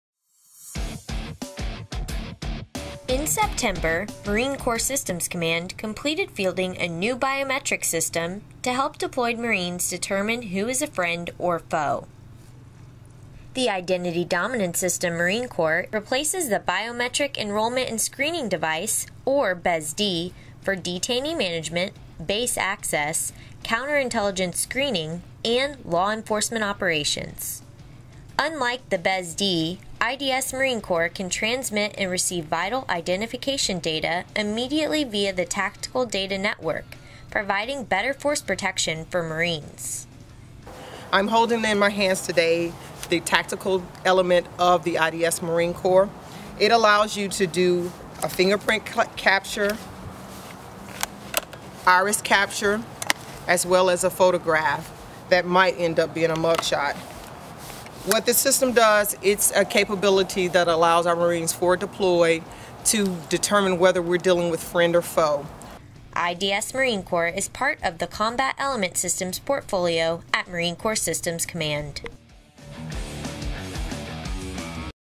Tactical Tuesday: IDS-MC
In September, we completed fielding of the Identity Dominance System-Marine Corps to help deployed Marines better determine who is friend of foe.